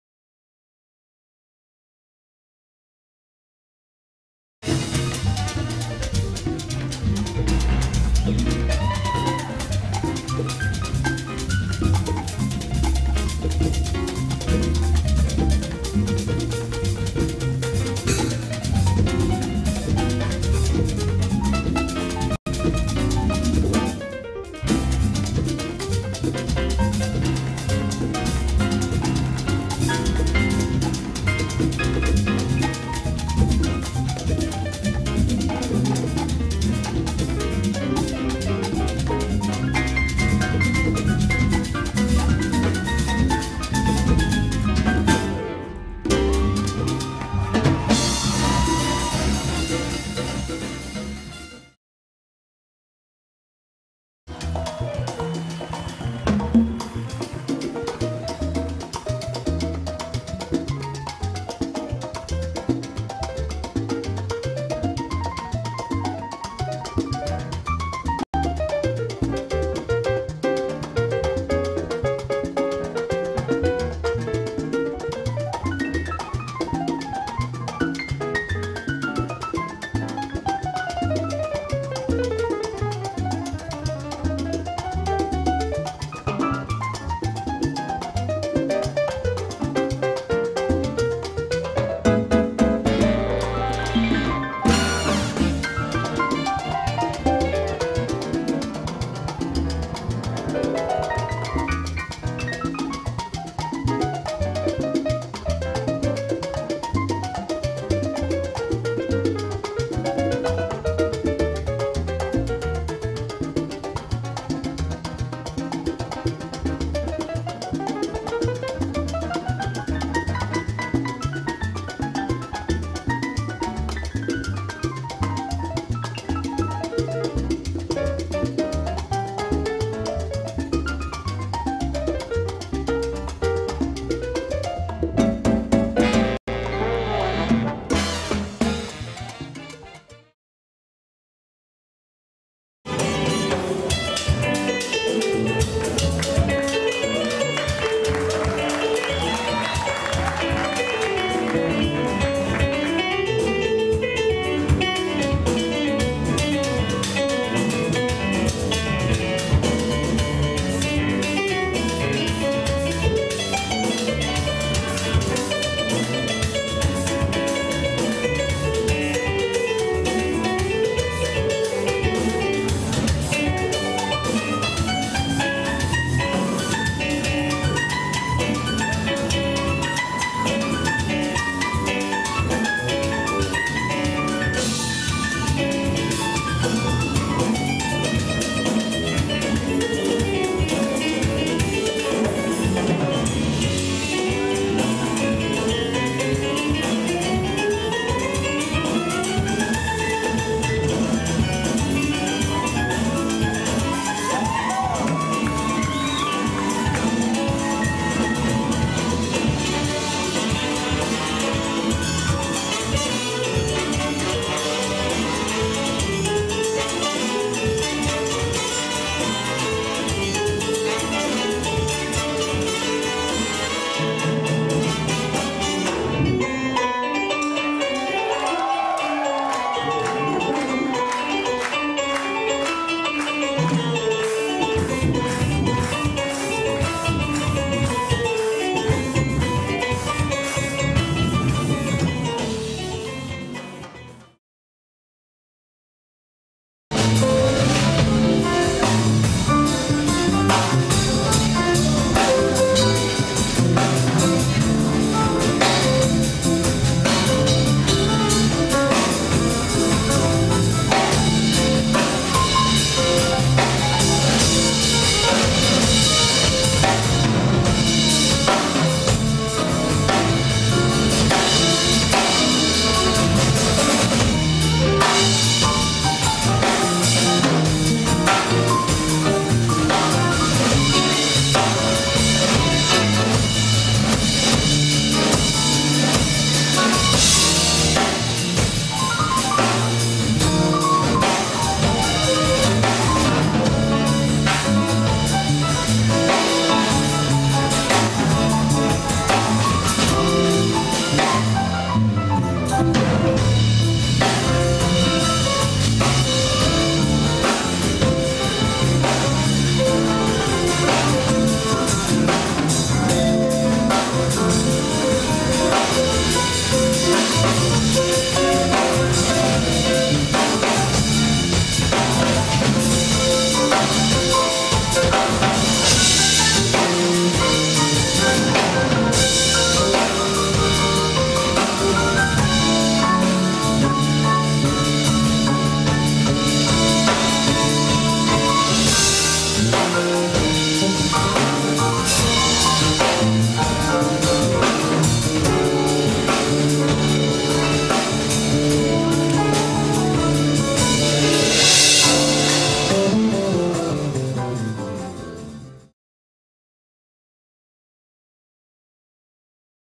WHILE ON TOUR - BOSTON, PUERTO RICO, MADAGASCAR & BEIJING!
A few of my solos throughout my career, during my visit to some amazing places and venues.